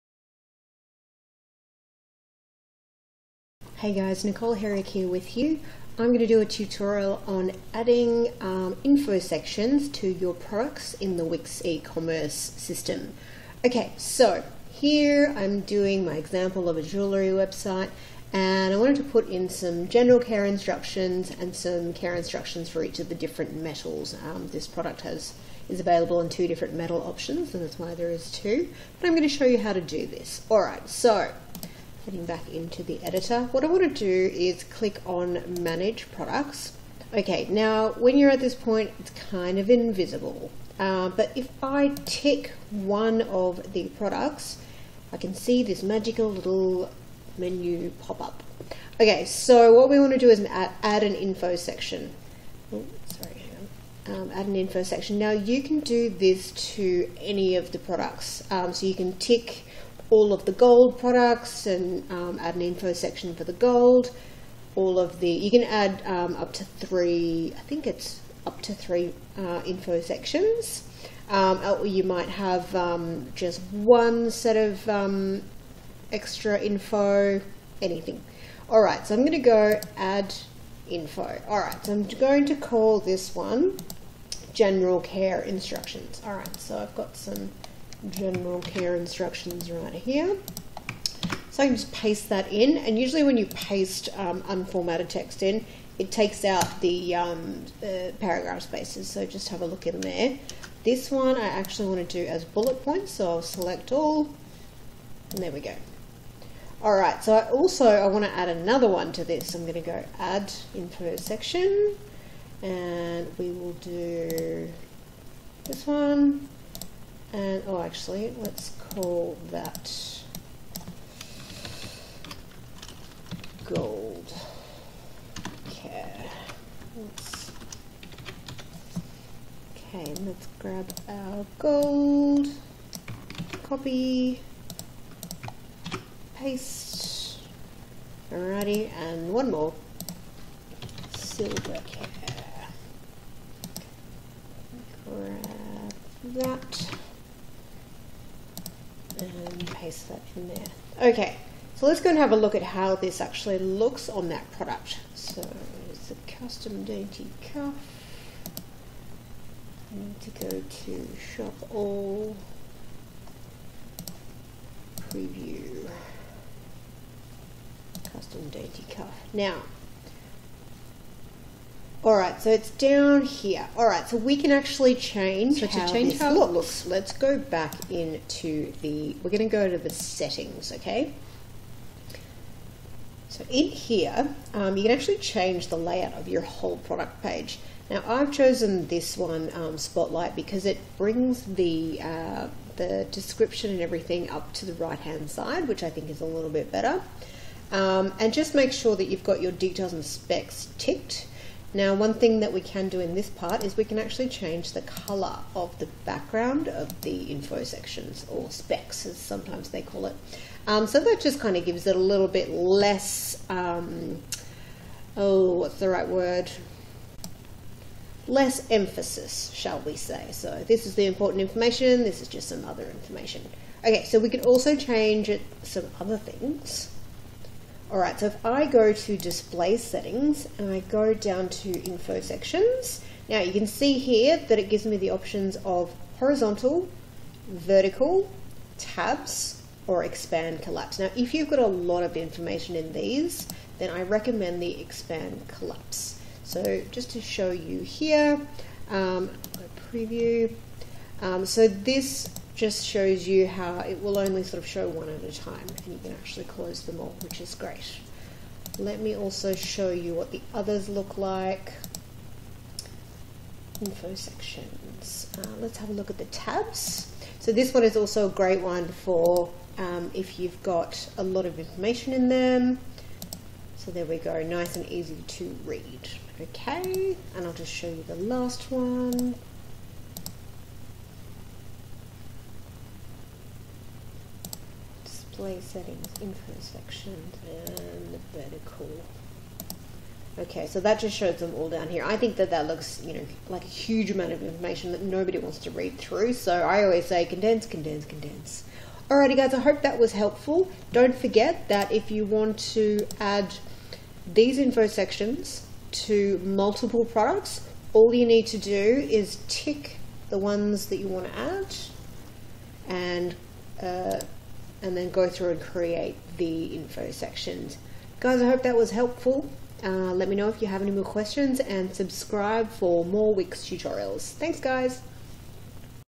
HOW TO ADD INFO SECTIONS TO MULTIPLE PRODUCTS - in Wix Ecommerce websites
Wix allows you to add info sections to multiple products at the same time. It's a much faster way of adding things like washing instructions, care info and other tidbits. This tutorial also shows you how to change the style of the info sections including tabs, expandable sections and columns.